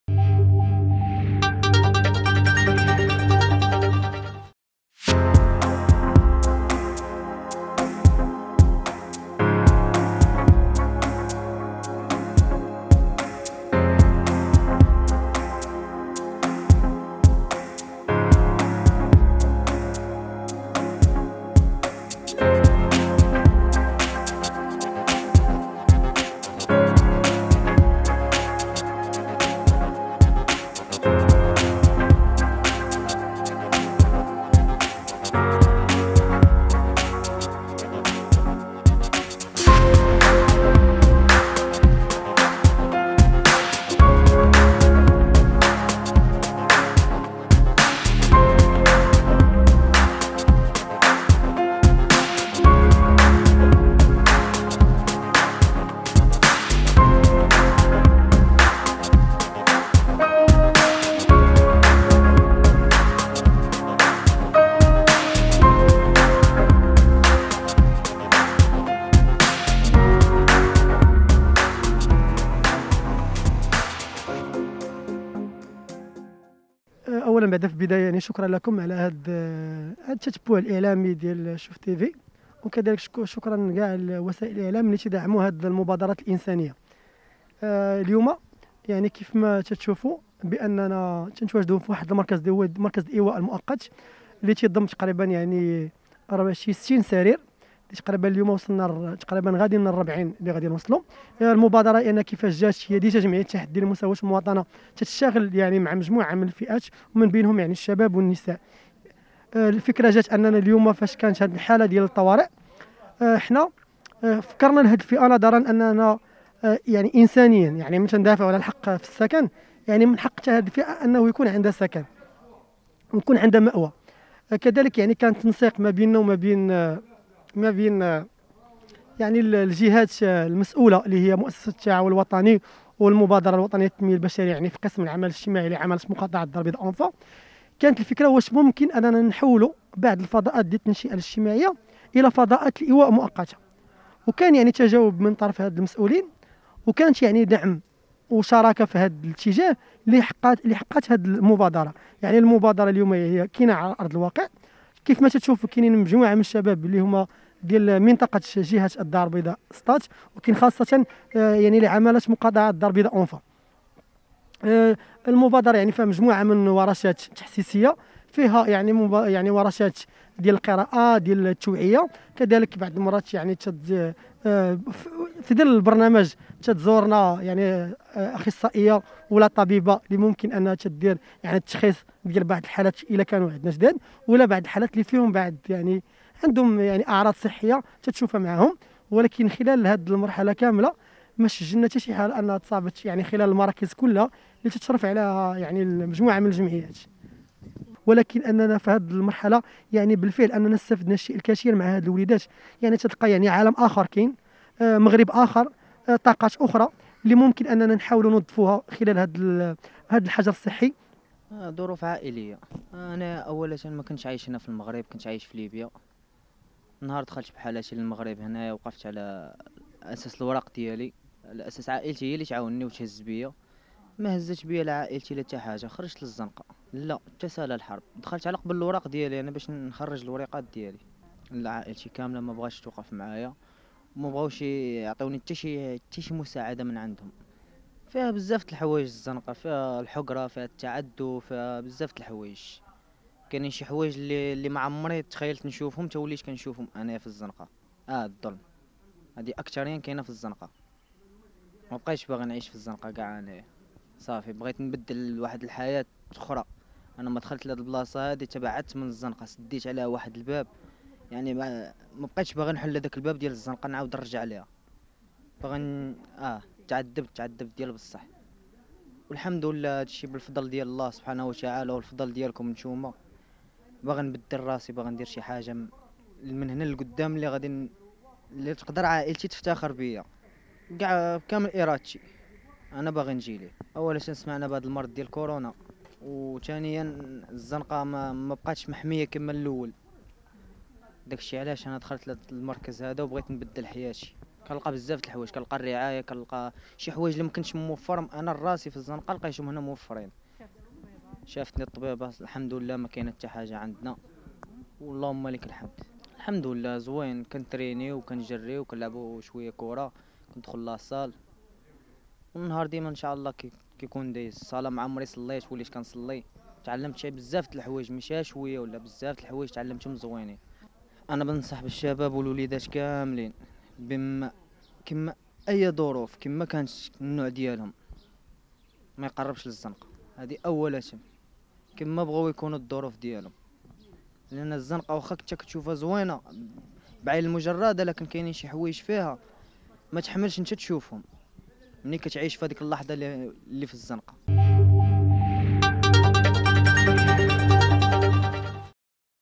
بادرة إنسانية فزمن كورونا..جمعية تتكلف برعاية المتشردين بالبيضاء
قامت جمعية بجمع العديد من المتشردين لرعايتهم خلال أزمة كورونا.